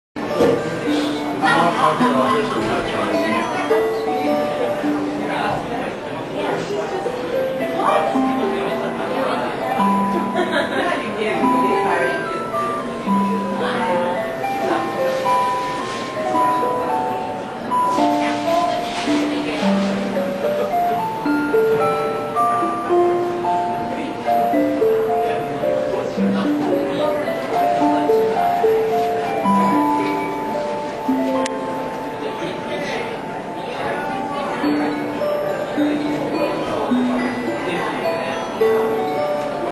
"Renaissance" Rhythm Magic Motion Clock 4MH788-R18
This is the "Renaissance" clock made by Rhythm. This video was downloaded from Springfield Clock Company’s website (exact date unknown).